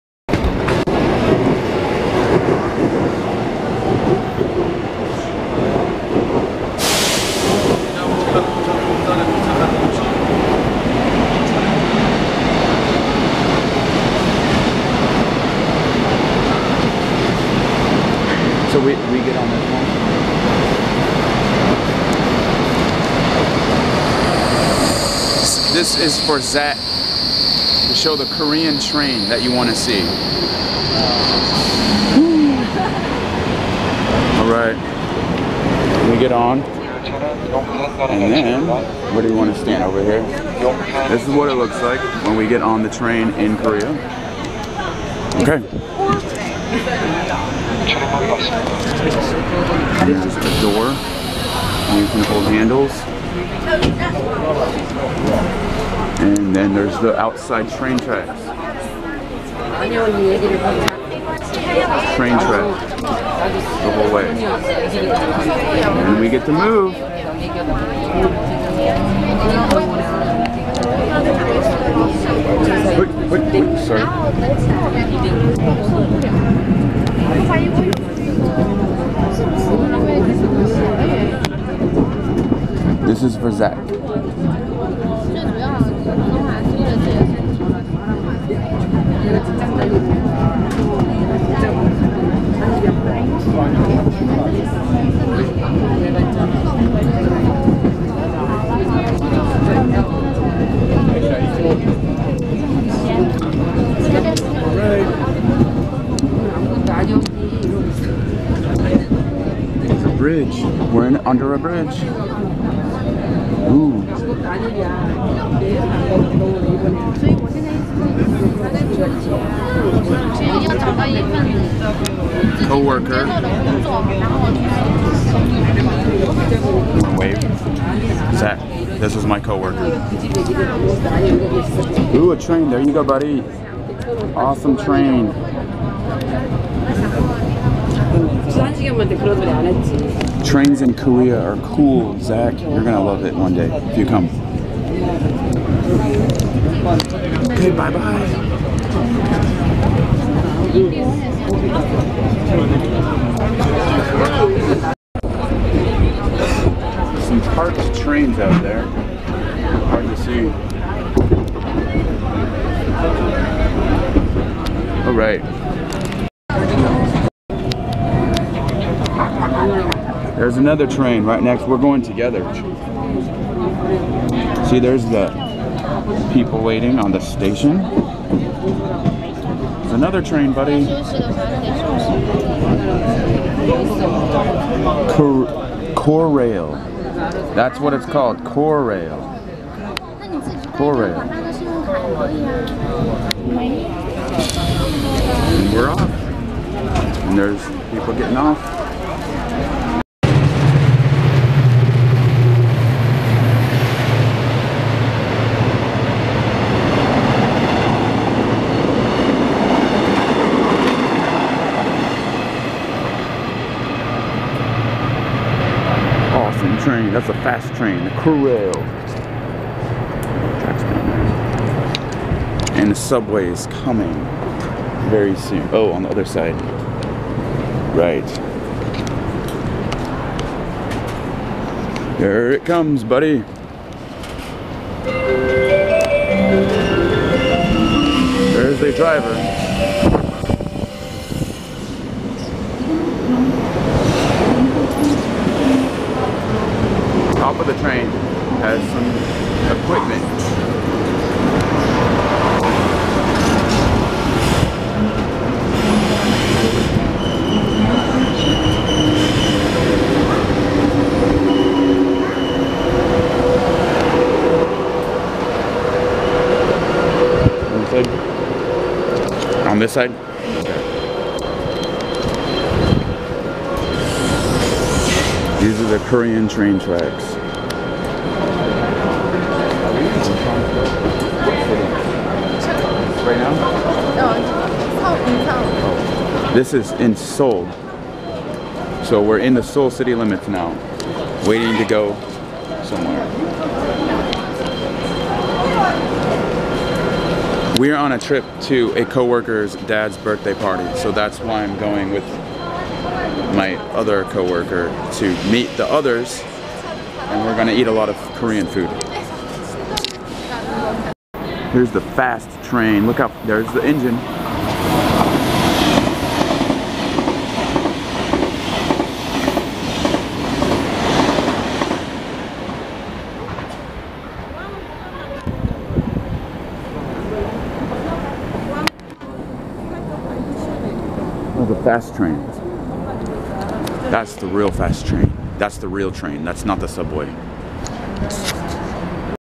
Korail Korean Subway Trains And Bullet Train Views in Bucheon and Seoul
I made this train video for my little nephew who is almost 7. He is obsessed, and I mean OBSESSED with trains. I told him when I go to Korea I will make a video showing Korean trains. They call this system Korail. I hope he enjoys it! And all you other crazy train nuts may get a kick too. Also anyone interested in Korea.